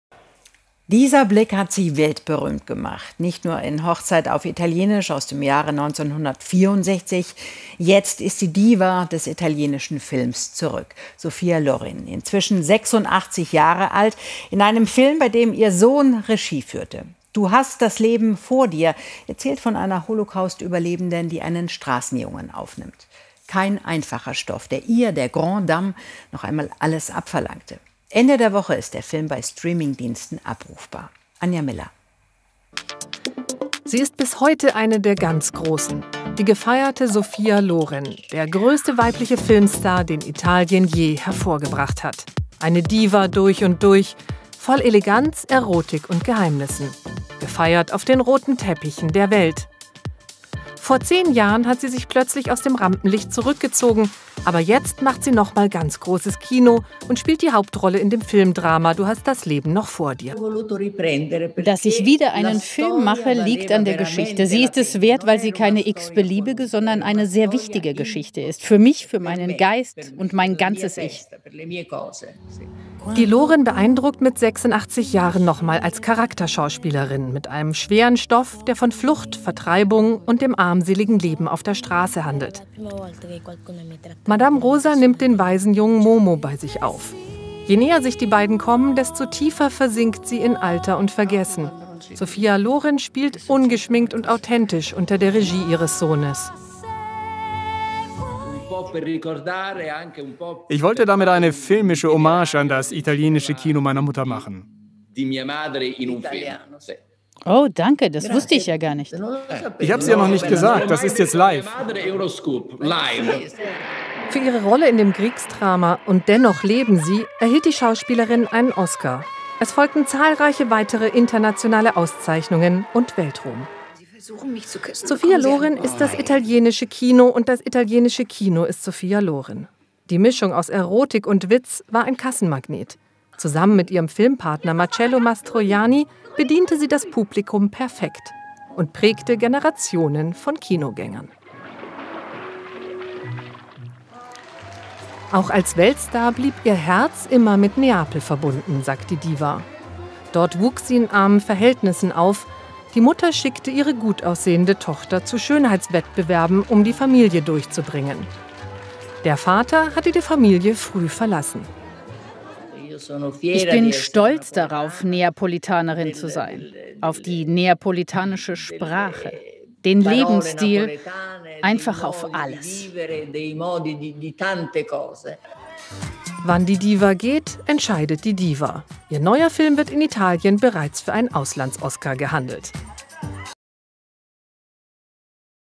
Comeback: Neuer Film mit Sofia Loren 86
Diva des italienischen Films, voll Eleganz, Erotik und ...

Video Comeback mit 86: Neuer Film mit Sofia Loren